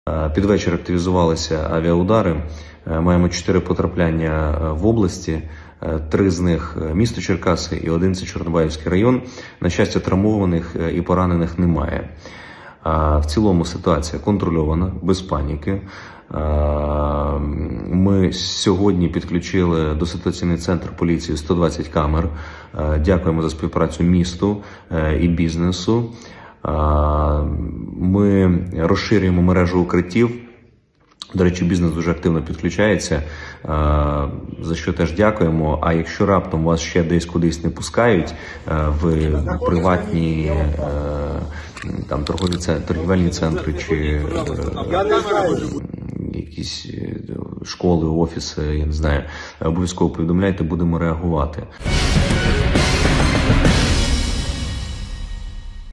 Черкасскую область обстреливают с воздуха - четыре попадания снарядов
Александр Скичко, губернатор Черкасской области: «Під вечір активізувались авіаудари, маємо 4 потрапляння в області, три з них – місто Черкаси і один – це Чорнобаївський район, на щастя, травмованих  і поранених немає, в цілому ситуація контрольована, без паніки. Ми сьогодні підключили до ситуаційний центр поліції 120 камер, дякуємо за допомогу місту і бізнесу, ми розширюємо мережу укриттів, до речі, бізнес теж підключається, за що теж дякую. А раптом вас ще десь не пускають у приватні центри, офіси – повідомляйте, будемо реагувати»